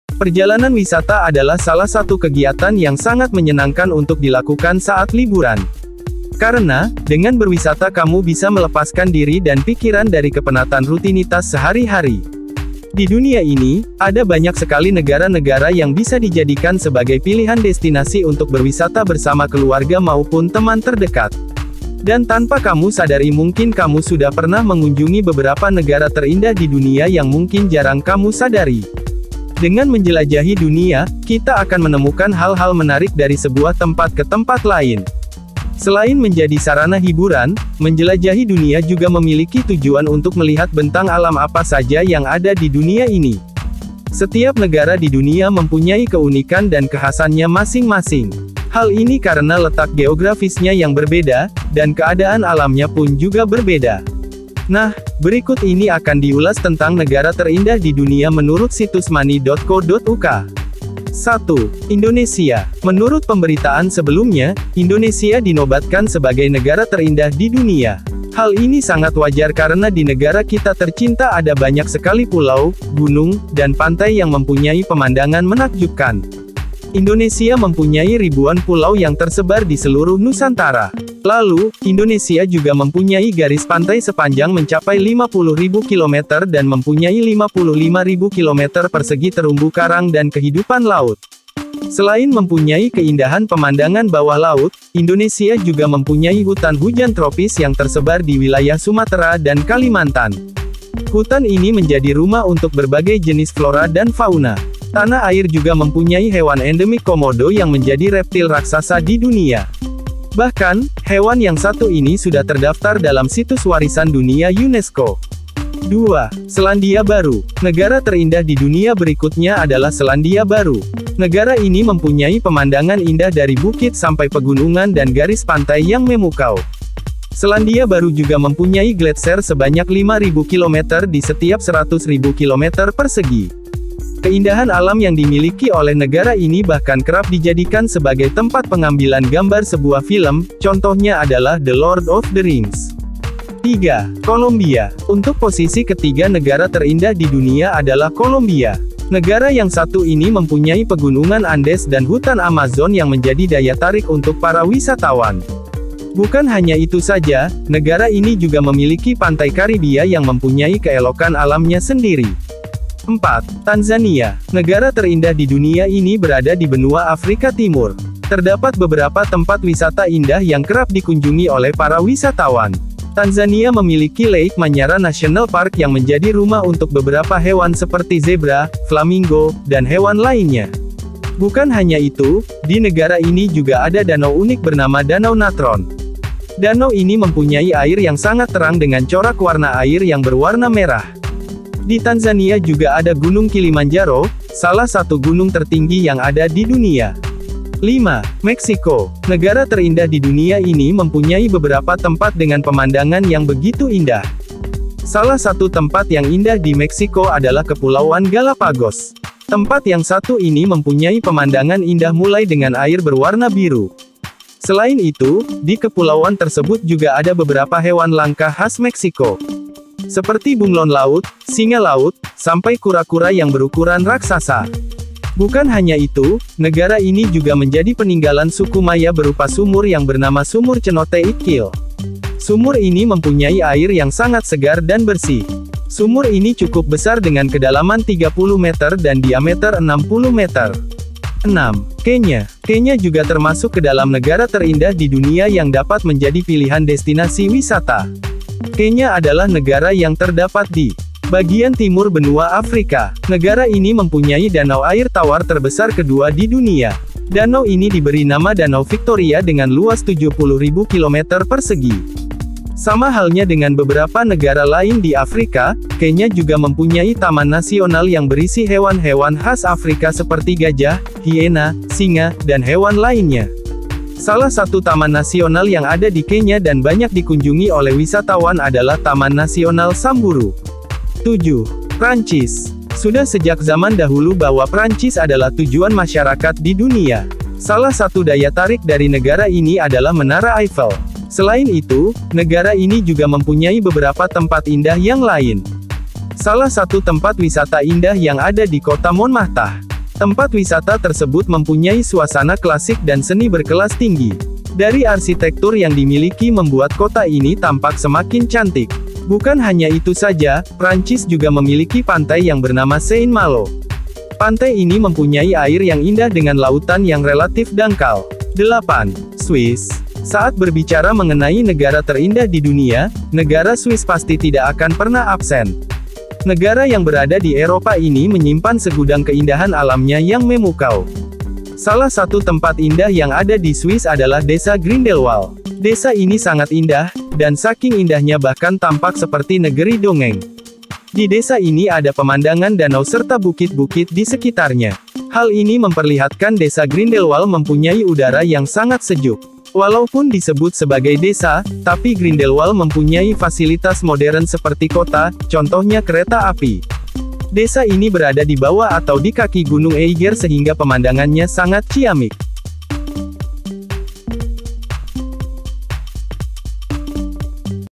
Negara terindah di dunia | negaraku, Eps.70
Selain di negaraku Indonesia, Di dunia ini, ada banyak sekali negara-negara yang bisa dijadikan sebagai pilihan destinasi untuk berwisata bersama keluarga maupun teman terdekat. Dan tanpa kamu sadari mungkin kamu sudah pernah mengunjungi beberapa negara terindah di dunia yang mungkin jarang kamu sadari.Setiap negara di dunia mempunyai keunikan dan kekhasannya masing-masing. Hal ini karena letak geografisnya yang berbeda, dan keadaan alamnya pun juga berbeda. Nah, berikut ini akan diulas tentang negara terindah di dunia.